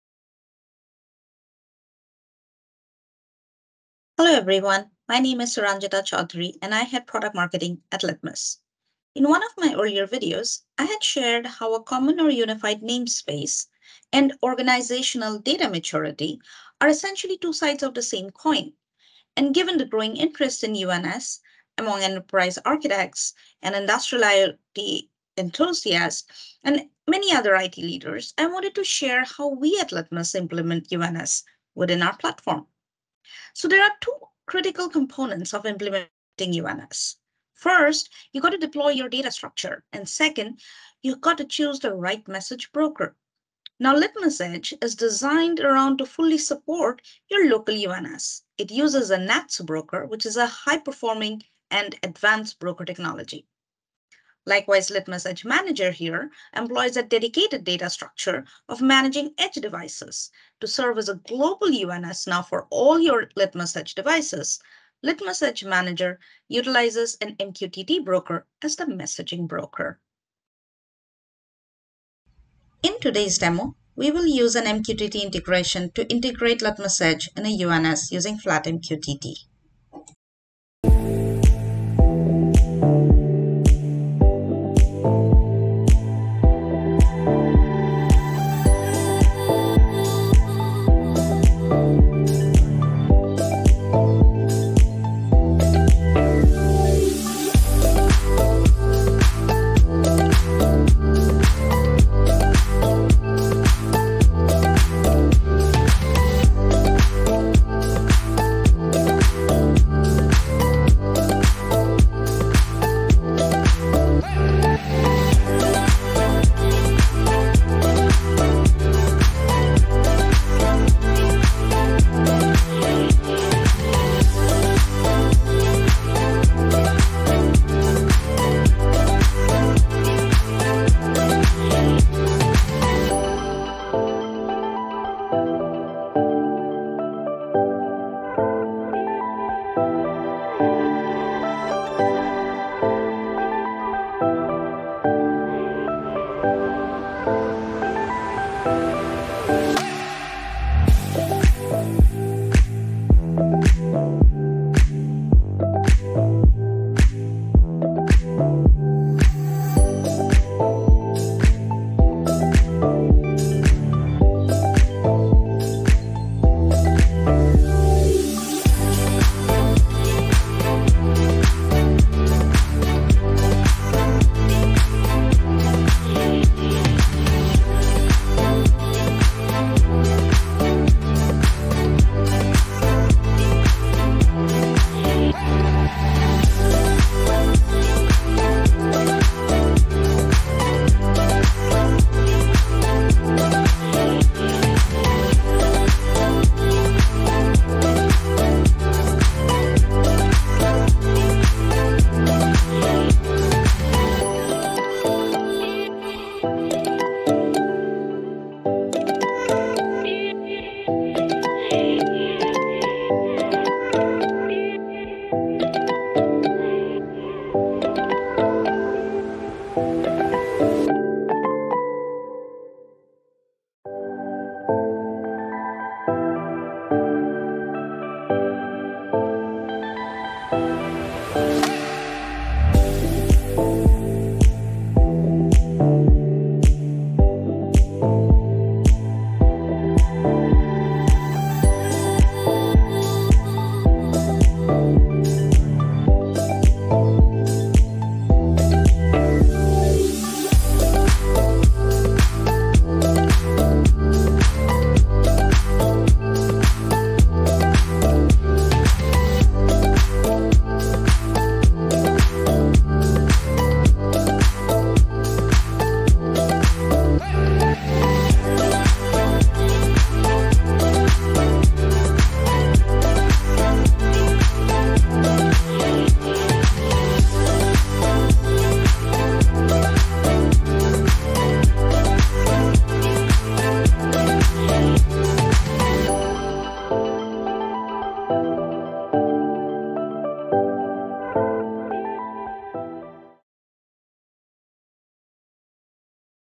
How to implement UNS using Litmus Edge
In one of my earlier videos, I had shared how a common or unified namespace and organizational data maturity are essentially two sides of the same coin. And given the growing interest in UNS among enterprise architects, IIoT enthusiasts, and IT leaders, I wanted to share how we at Litmus implement UNS within our platform. There are 2 critical components of implementing UNS: first, defining your data structure and second choosing the right message broker.
Litmus Edge is designed around to fully support your local UNS. It uses a NATS broker - a high-performing and advanced broker technology.
Likewise, Litmus Edge Manager employs a dedicated data structure for managing edge devices. To serve as a global UNS for Litmus Edge devices, Litmus Edge Manager utilizes an MQTT broker as the messaging broker.

In today’s demo, we will use an MQTT integration to integrate Litmus Edge in a UNS using Flat MQTT.